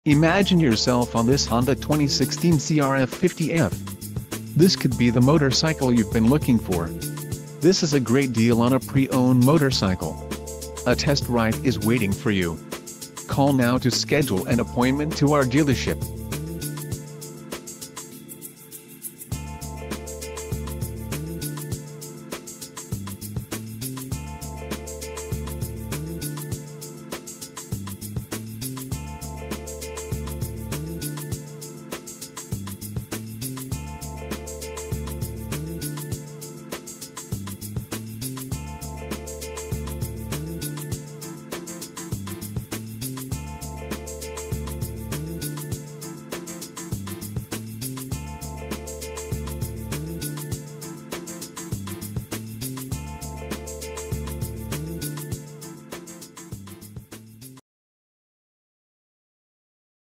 2016 Honda CRF50F
2016 Honda and #174; CRF50FThe World's Best Beginner Bike.
How many thousands of beginners have had their first taste of motorcycling on a Honda 50? Hundreds of thousands, for sure. And it and #8217;s easy to understand why. The CRF and #174;50F offers up an unbeatable combination: Its tough, reliable engine develops smooth, easy-to-use power; three-speed transmission with automatic clutch; and tough suspension that stands up to youthful enthusiasm. And for adults, the CRF and #174;50F makes sense too: an adjustable throttle limiter lets you tailor the power to the appropriate skill level, and the proven reliability means more time riding and less time taken up by maintenance and repairs.

Be A Responsible Rider: All parents need to consider their youngsters' age, size, ability and maturity before allowing them to ride.